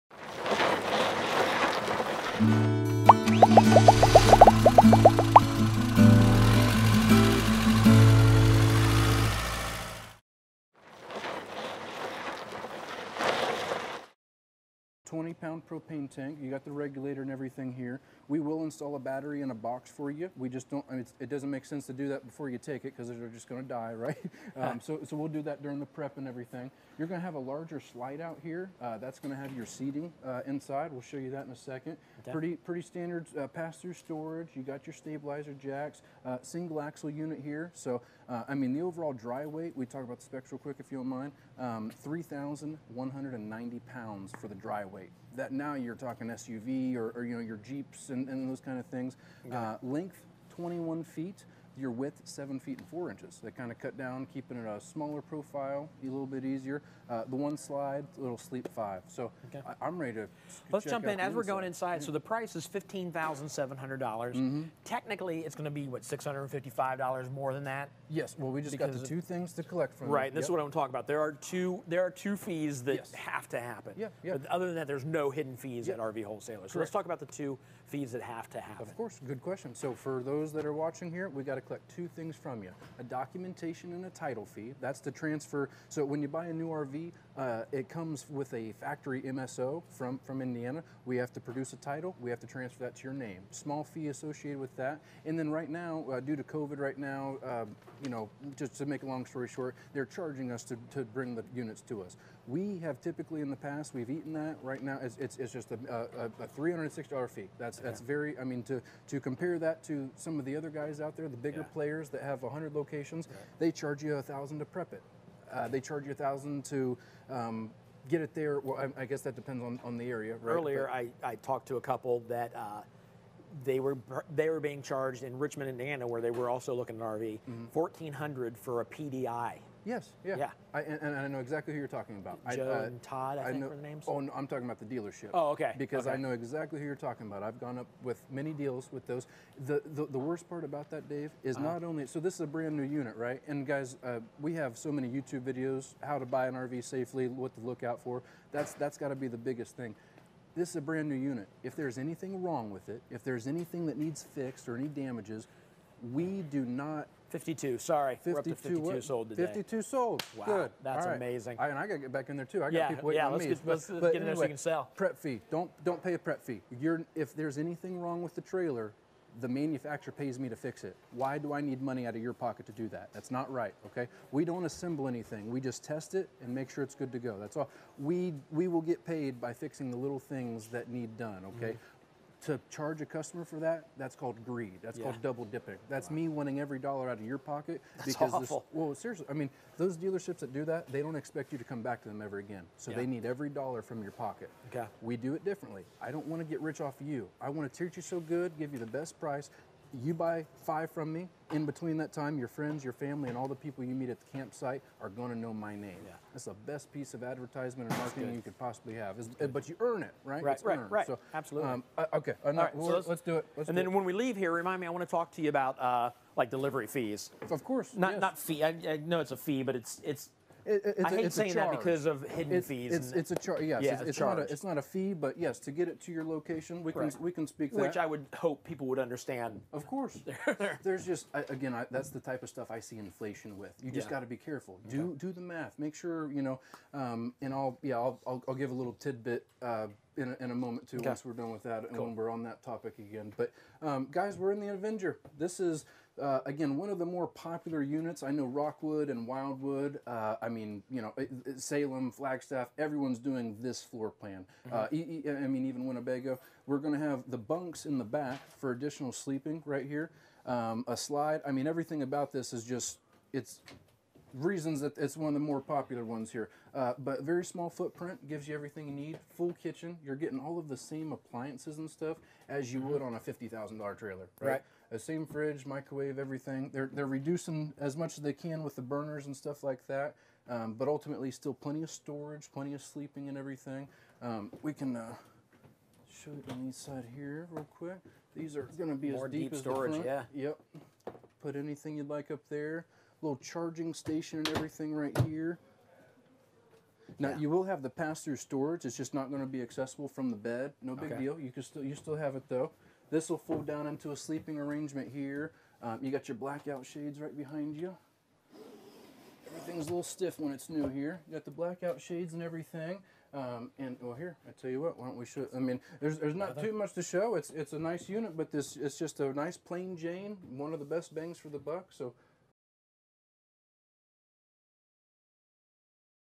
2021 Avenger LT 17BHS Travel Trailer Walk-Through
Check out this walk-through with our sales consultant Josh Durnell and Virtual RV Show host David McCreary on the 2021 Avenger LT 17BHS by Prime Time RV.

This unit was showcased in our monthly Virtual RV Show and any show price discussed or shown in the video may not be accurate to what you will see on the website. The price will also not be the same when discussing prices with salespeople. This is a show exclusive price.